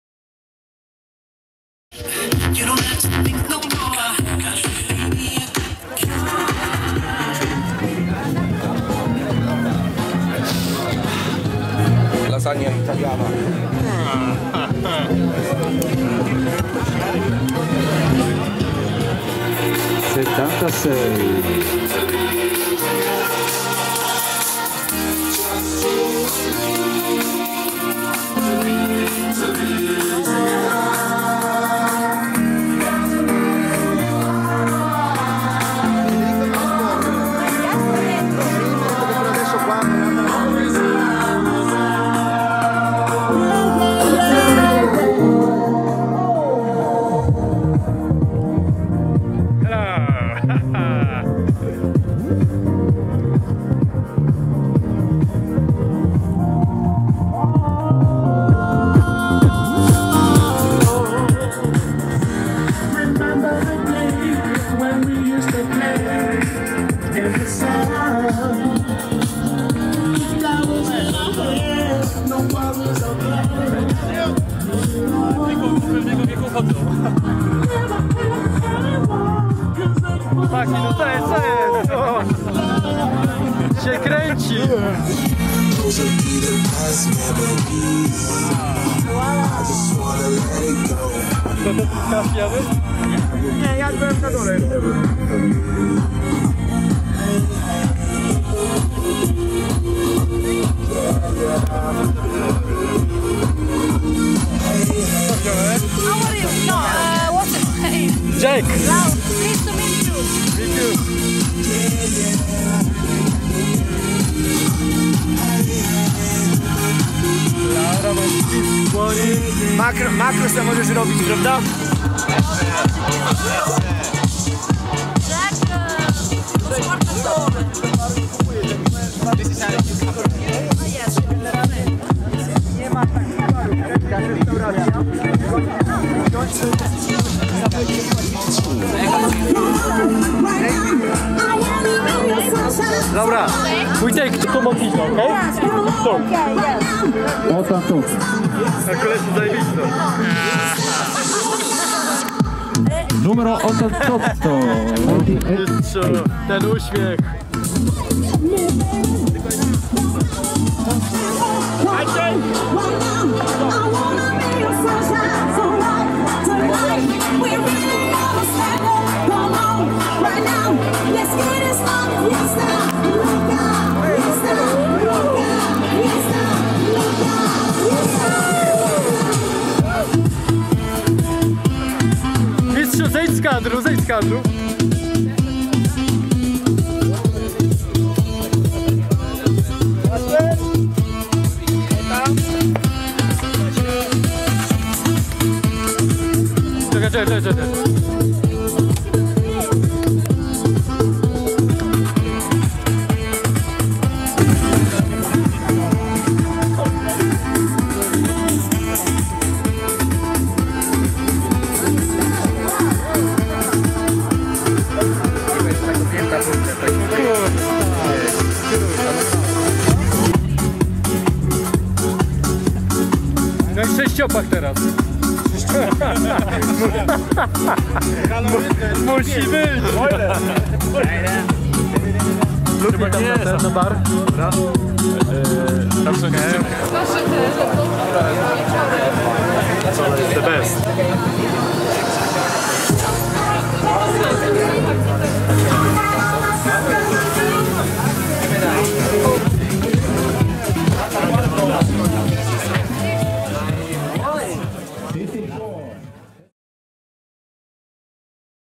Ravascletto Part 1. Ski Italia Heliasz HD Movie
Ravascletto 2011 . Zimowa słoneczna Italia.
Wyjazd zorganizowany przez Fujilm i Szkołę Zdrowia. Camera Fujifilm Finepix EXR 550
Movie by Heliasz